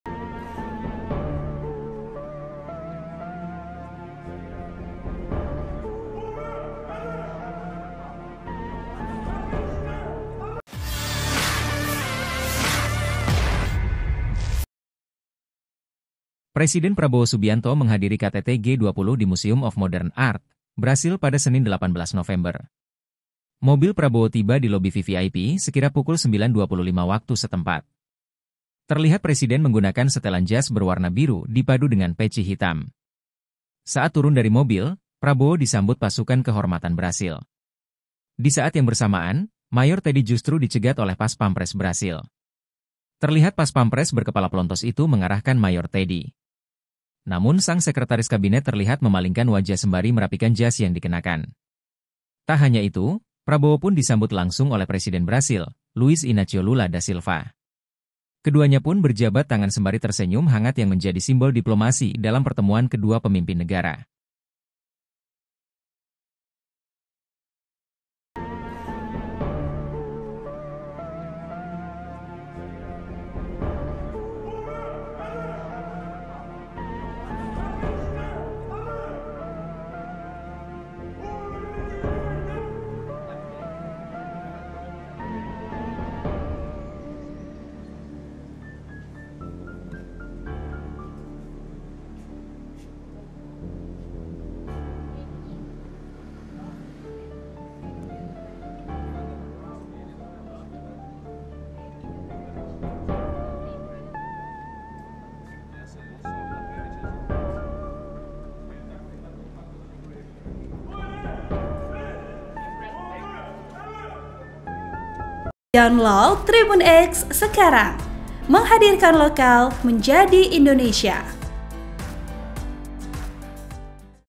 Prabowo Disambut Pasukan Kehormatan di KTT G20, Mayor Teddy Dicegat Paspampres.
VP: Roly Maundeng

Presiden Prabowo Subianto menghadiri KTT G20 di Museum of Modern Art, Brasil pada Senin (18/11).

Mobil Prabowo tiba di lobi VVIP sekira pukul 09.25 waktu setempat.

Terlihat presiden menggunakan setelan jas berwarna biru dipadu dengan peci hitam.

Saat turun dari mobil, Prabowo disambut pasukan kehormatan Brasil.

Di saat yang bersamaan, Mayor Teddy justru dicegat oleh paspampres Brasil.

Terlihat paspampres berkepala plontos itu mengarahkan Mayor Teddy.

Namun sang Sekretaris Kabinet terlihat memalingkan wajah sembari merapikan jas yang dikenakan.